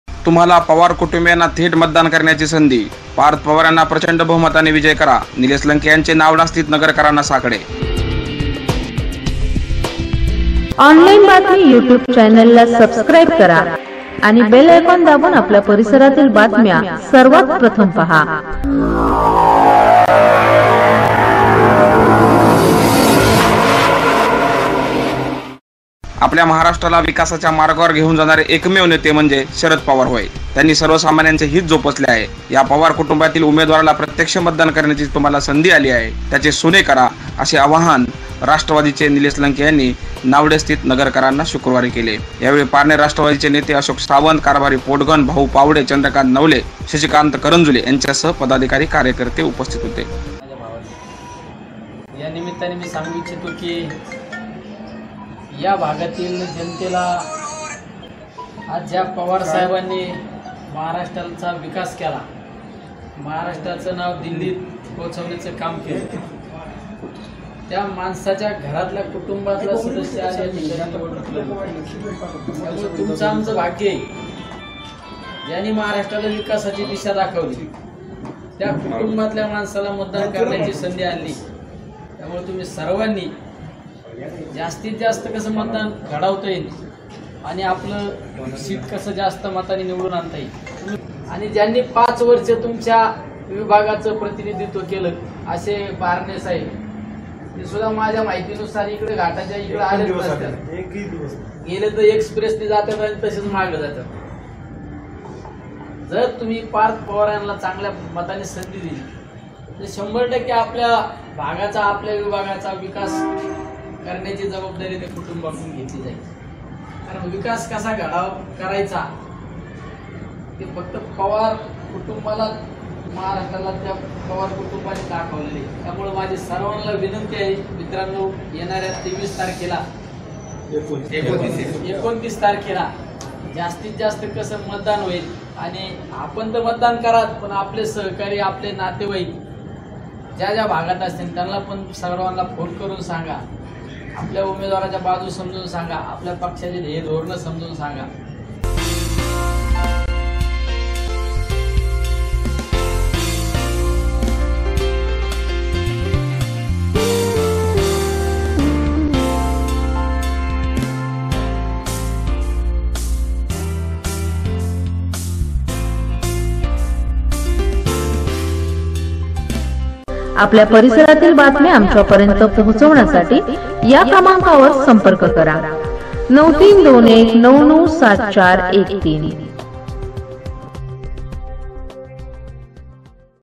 पवार कुटुंबियांना थेट मतदान करण्याची संधी! पार्थ पवार यांना प्रचंड बहुमताने विजय करा
निलेश लंके यांचे नावडे स्थित नगरकरांना साकडे साकडे
पनवेल:  आपल्या महाराष्ट्राला विकासाच्या मार्गावर घेऊन जाणारे एकमेव नेते म्हणजे शरद पवार होय. त्यांनी सर्वसामान्यांचे हित जोपासले आहे. या  पवार कुटुंबातील उमेदवाराला प्रत्यक्ष  मतदान करण्याची तुम्हाला संधी आली आहे. त्याचे सोने करा असे आवाहन राष्ट्रवादीचे नेते निलेश लंके यांनी नावडे स्थित नगरकरांना शुक्रवारी केले. यावेळी पारनेर राष्ट्रवादीचे नेते अशोक सावंत, कारभारी पोटघन, भाऊ पावडे, चंद्रकांत नवले, शशिकांत करंजुले यांच्यासह पदाधिकारी कार्यकर्ते उपस्थित होते.